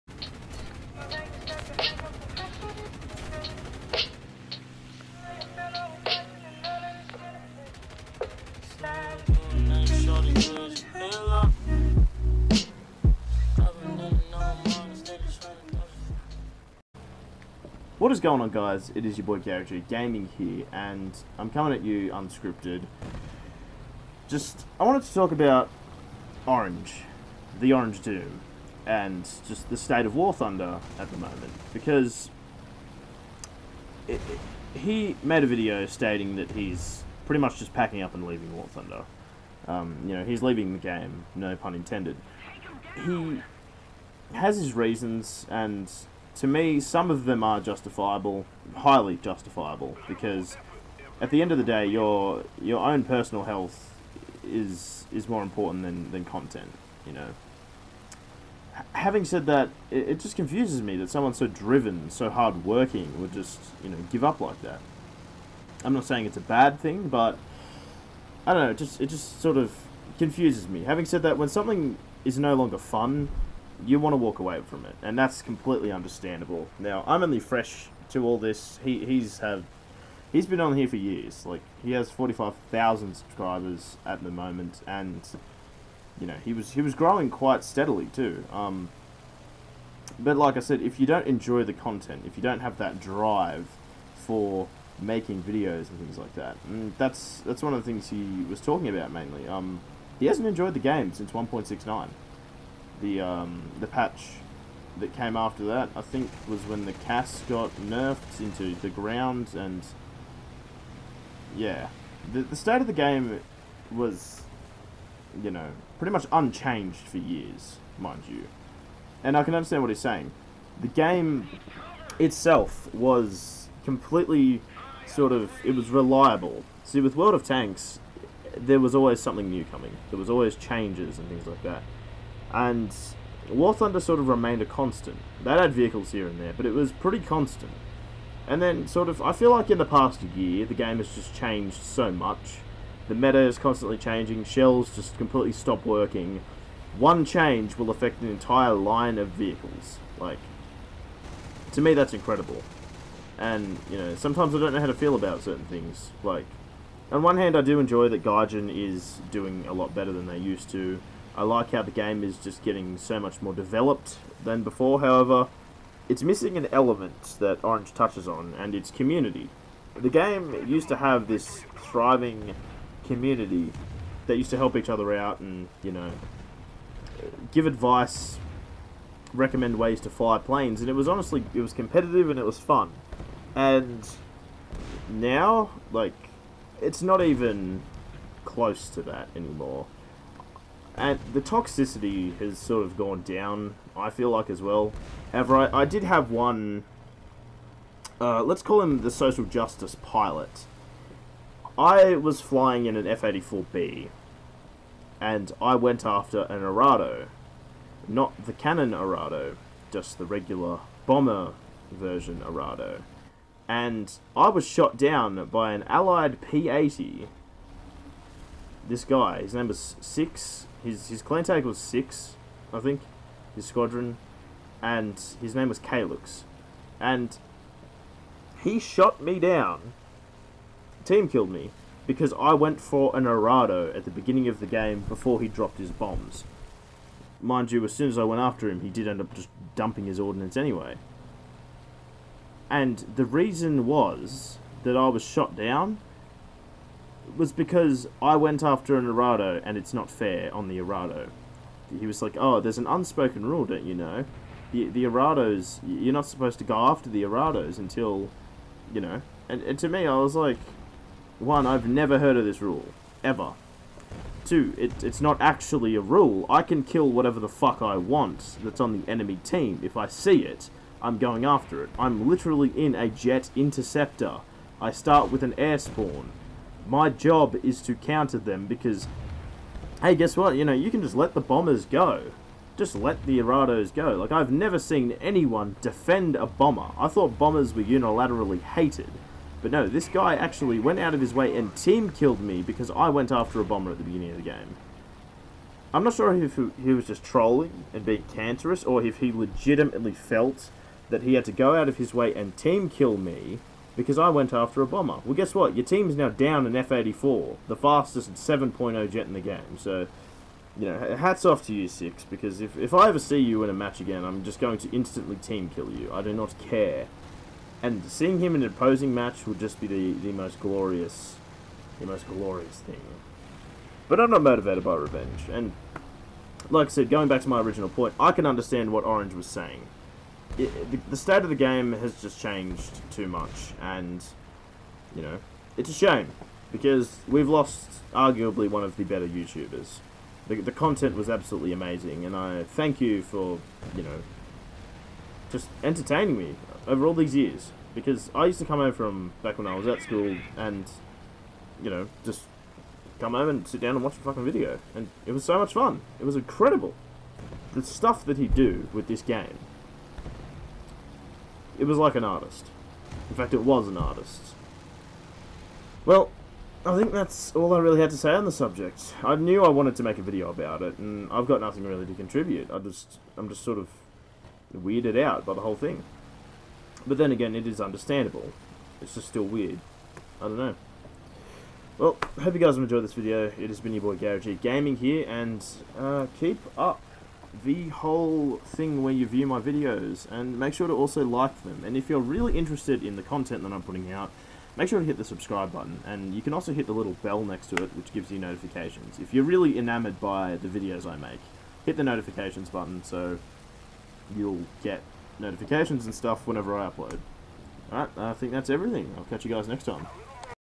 RIP theOrangeDoom (2013-2018)
The God of War has fallen, of course irl Tim (Orange) is fine, but he's no longer making War a Thunder videos nooooooooooooooooooooo. I'll always remember the streams and the fun times. Hope things go well for you Orange